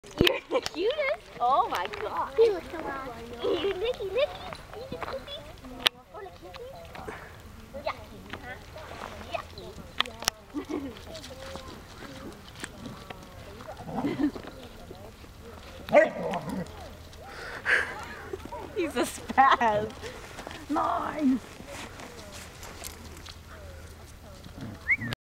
Snow Time
The pack goes out for a romp in the snow